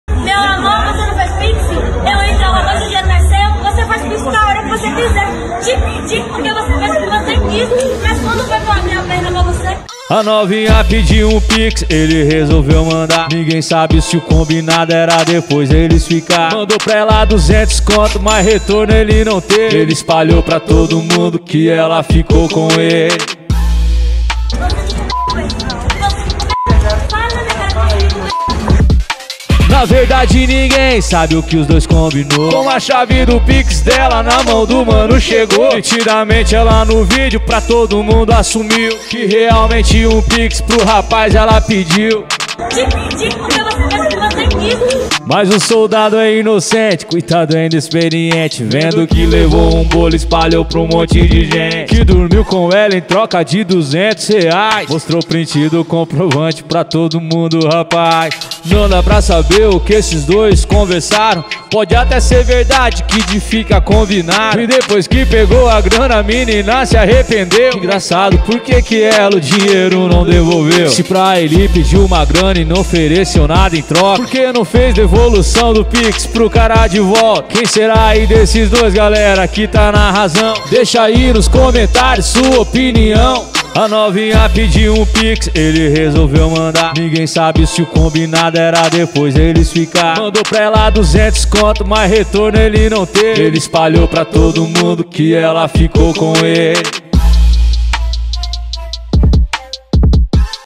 RAPAZ FEZ UM PIX DE 200,00 PRA AGRADAR A MOÇA!
A mulher aceitou o dinheiro, não devolveu pro rapaz e de quebra não ficou com ele kkkk

Créditos @ictvnoticias
Link do vídeo completo